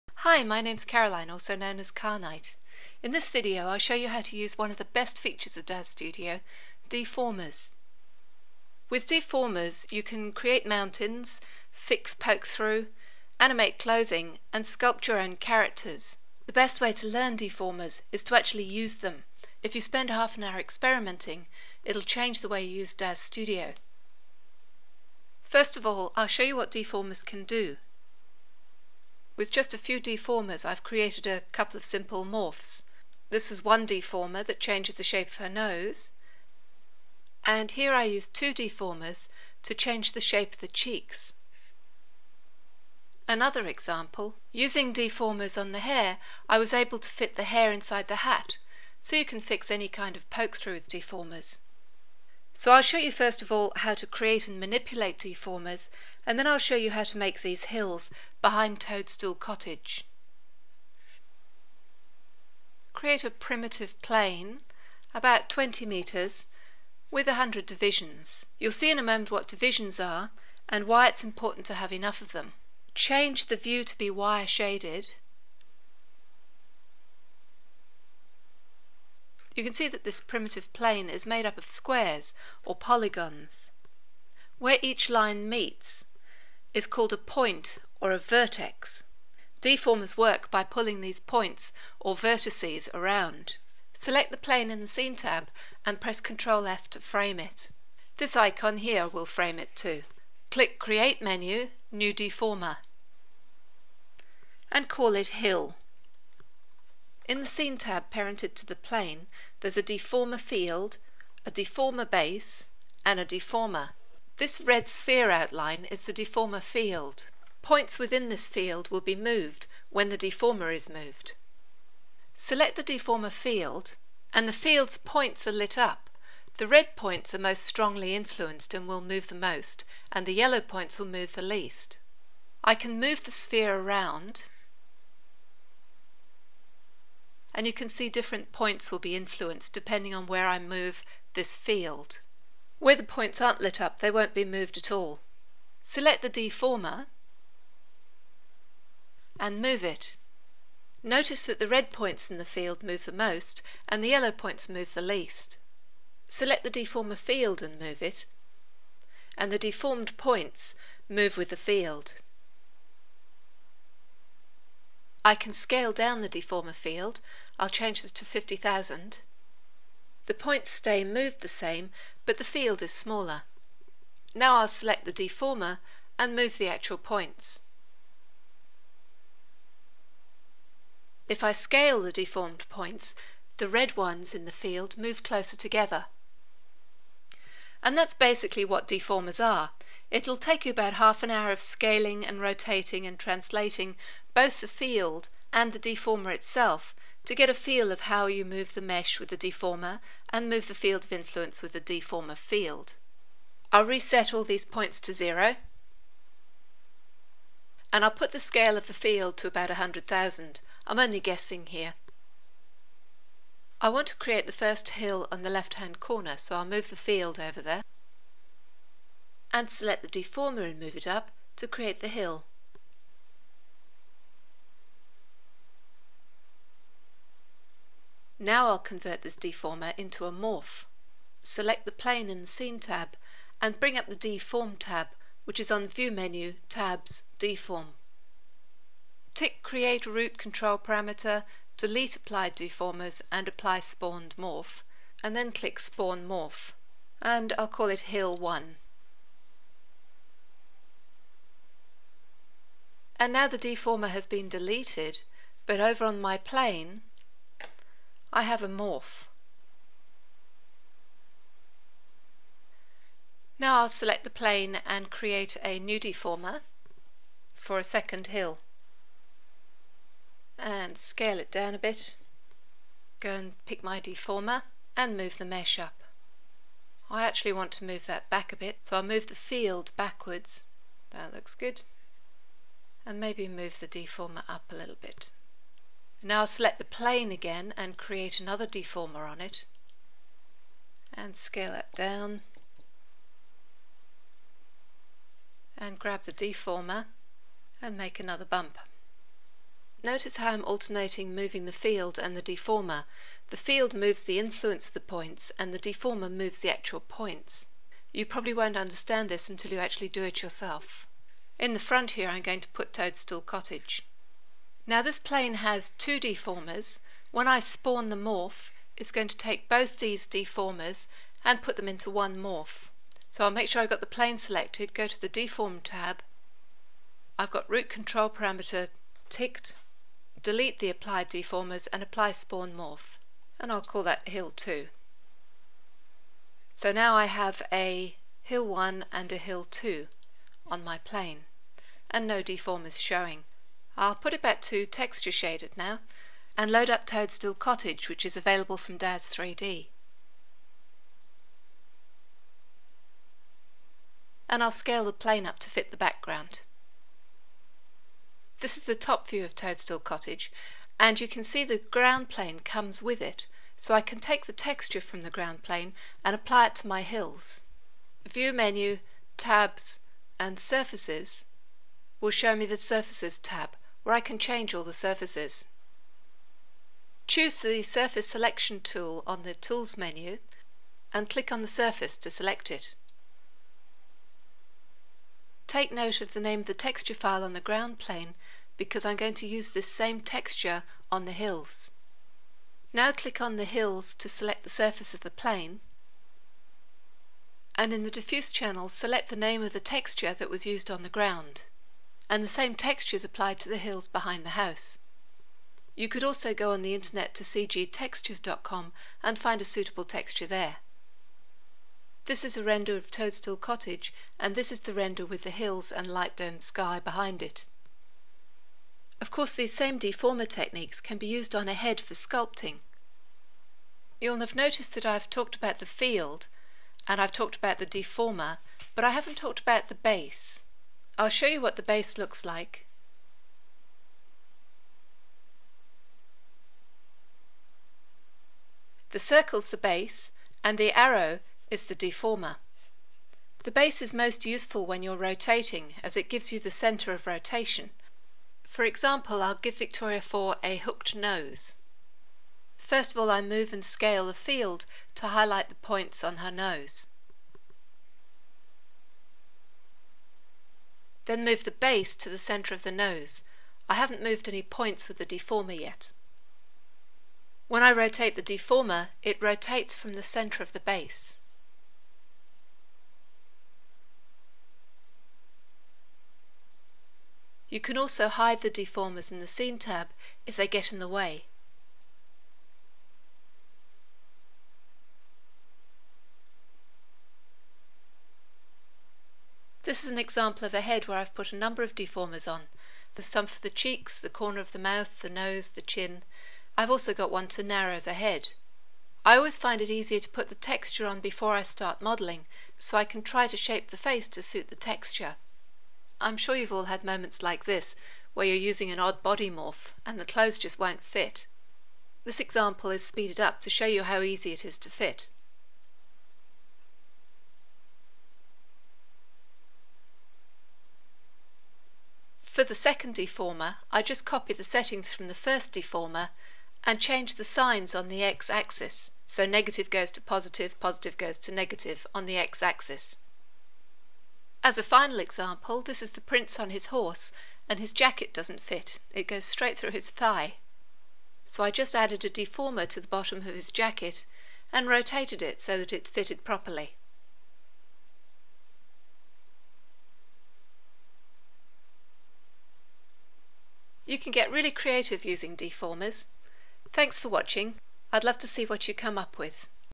D-Formers in DAZ Studio
This is a tutorial on how to use D-Formers in DAZ Studio. You can create your own morphing terrains, fix poke-through and create your own custom morphs.